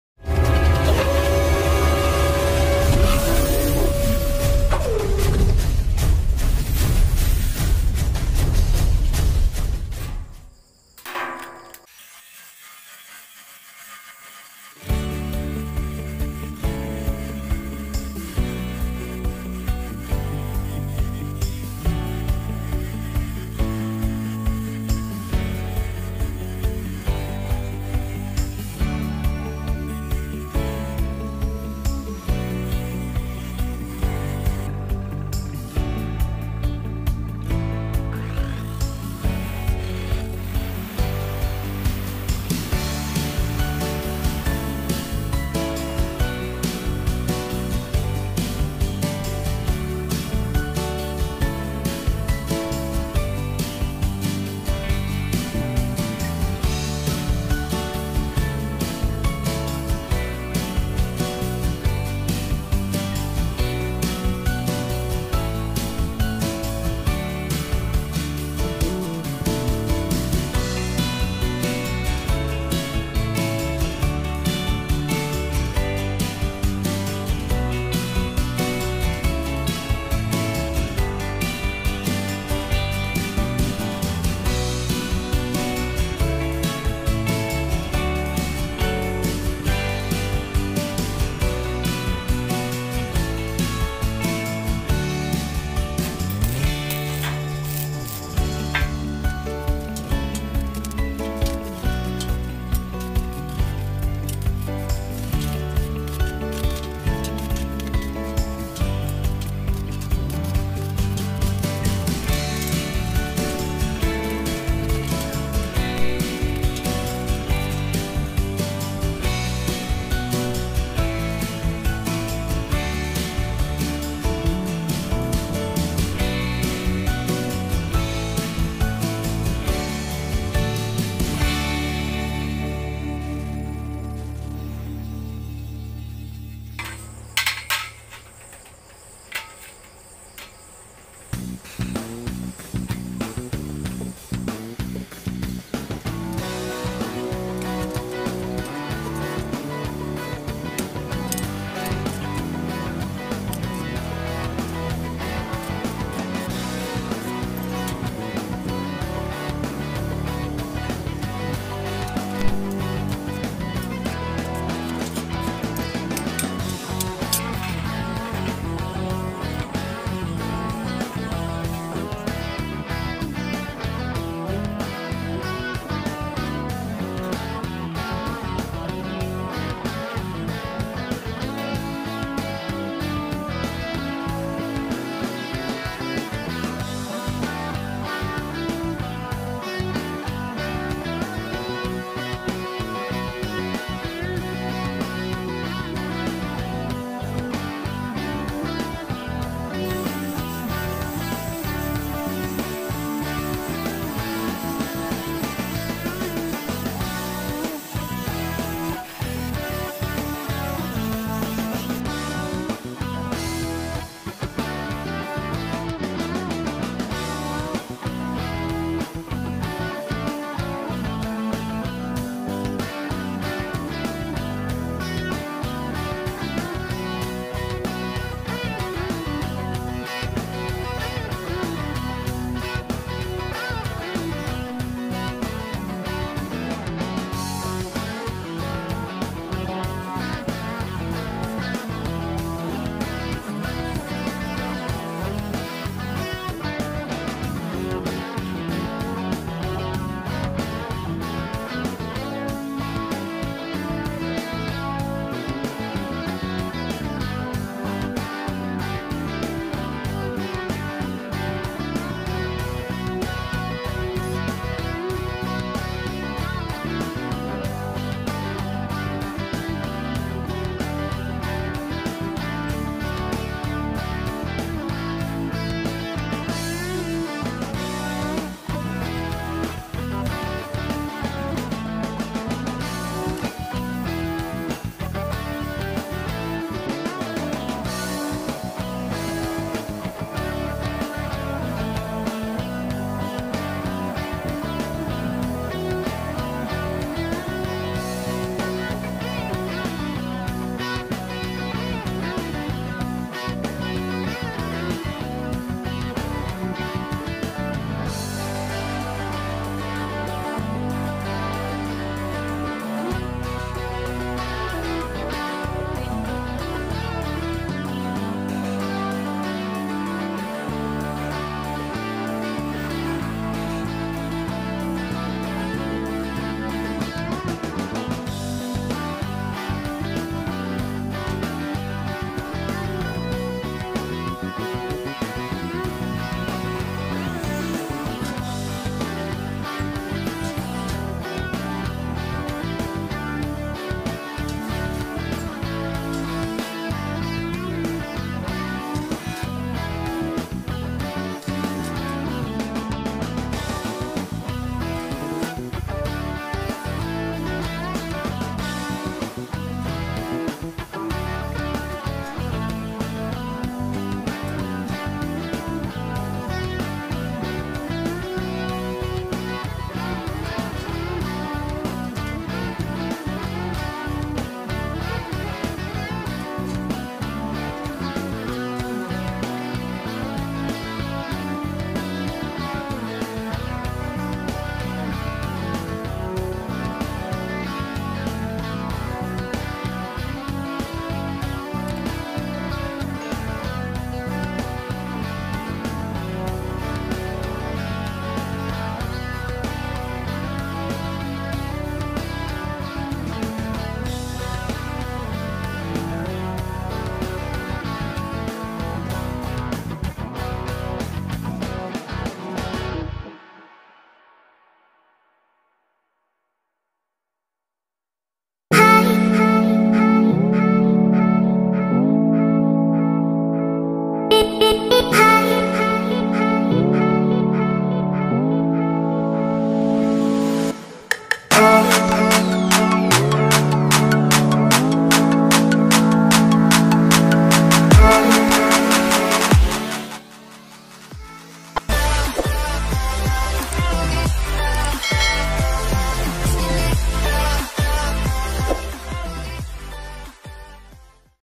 Hand sanitizer // Meja westafel steanlis, cuci tangan dari rangka besi holo
Assalamualaikum,  terimakasih yang selalu support channel pemula ini, seperti teman ketahui akhir akhir ini wabah virus sedang merajalela disekitar kita, maka itu kita harus waspada dengan virus tersebut, tapi di balik semua itu ada setitik rejeki bagi sebagian kalangan termasuk bengkel las, dengan membuat westafel steanlis cuci tangan dari rangka besi holo, ok langsung saja selamat menyaksikan 😁🙏